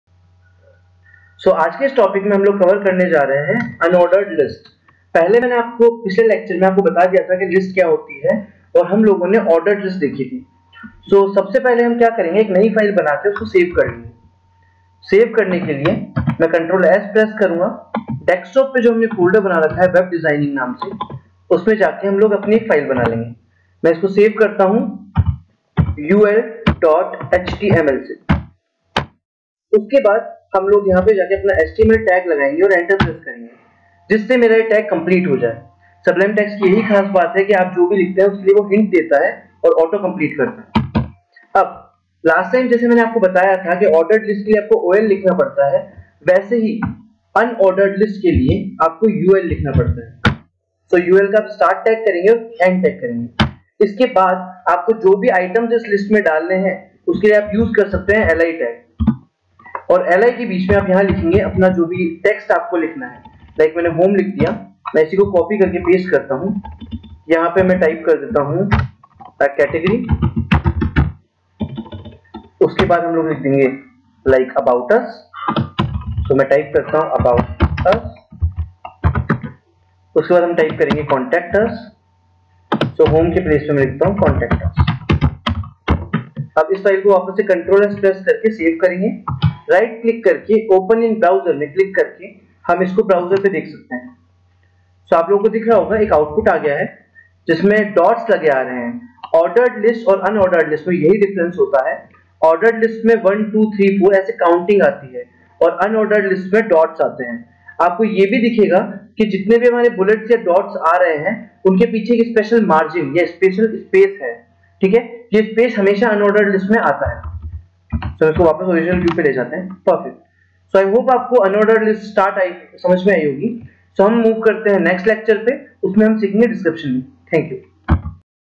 Unordered List in HTML (Hindi)
In this tutorial i will talk about Unordered List in HTML

An unordered list is a collection of related items that have no special order or sequence. This list is created by using HTML ( ul ) tag. Each item in the list is marked with a bullet.